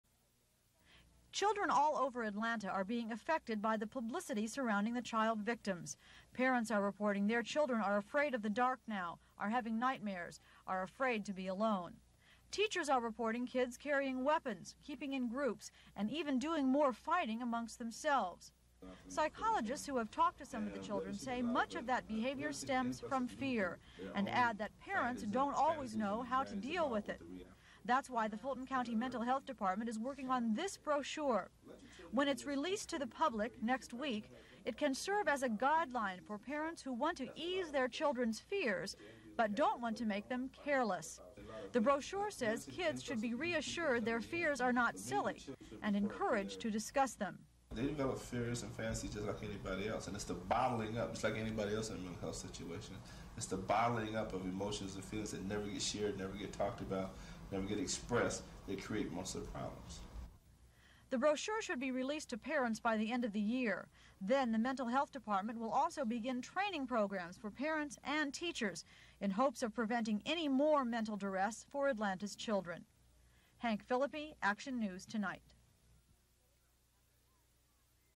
Fulton County Mental Health Dept Creates Brochure to Help with Atlanta Child Murder Anxiety 12/18/80
While several children play in a field, Hank Phillippi reports on increased reports of children being scared by the incessant publicity surrounding the Atlanta Child Murders. Phillippi also reports that child psychologists have prepared a report for parents that outline the best ways to ensure their children’s safety, without terrifying them.
Reporter: Phillippi, Hank